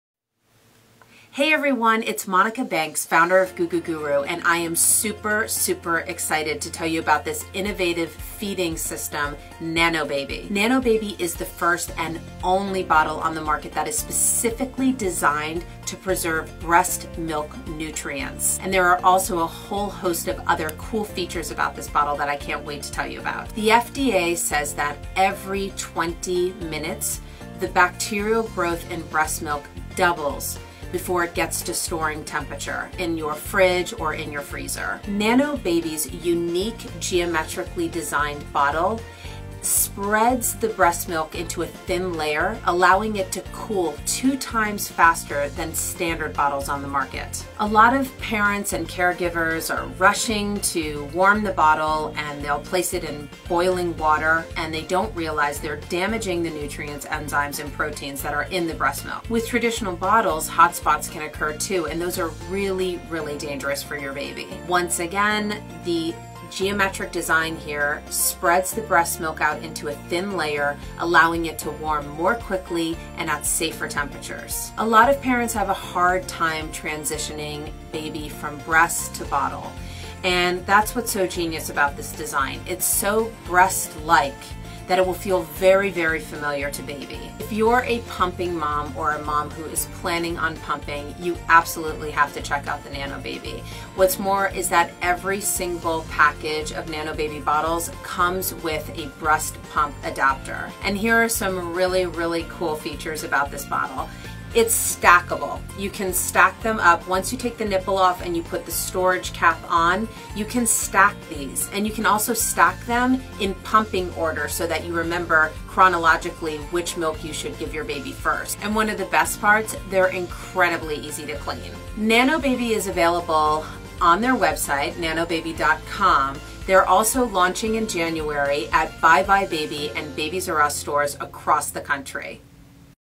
Gugu Guru - Nanobébé Baby Bottle Expert Review
Nanobébé is the first and only bottle designed to preserve breastmilk nutrients without compromising on mom’s convenience and style. Pumping mamas - this is your new must-have. In this video, Gugu Guru founder Monica Banks reviews the Nanobébé Breastmilk Bottle and explains the science behind how it works.